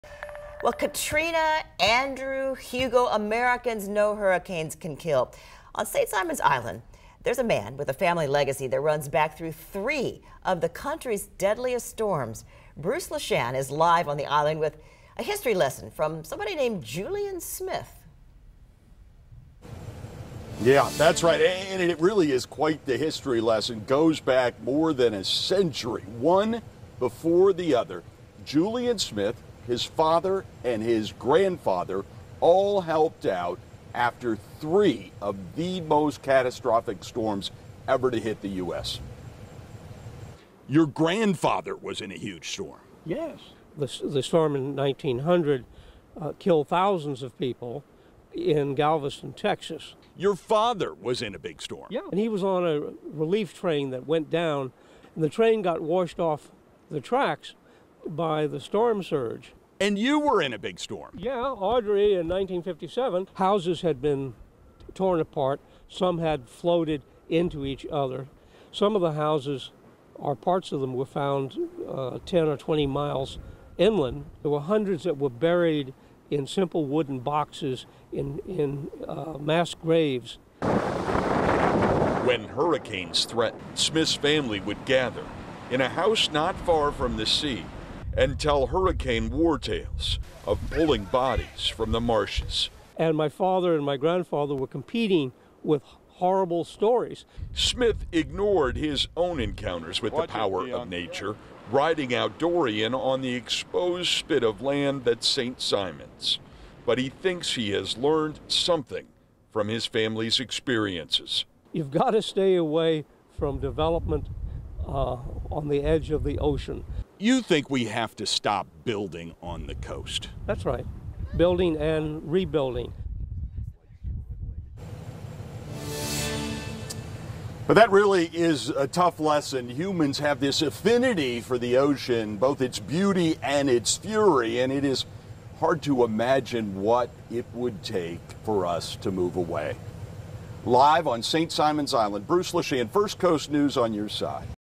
St. Simons man's family legacy includes surviving, helping during deadly storms
On St. Simons Island, there’s a man with a family legacy that runs back more than a century through three of the country’s deadliest storms.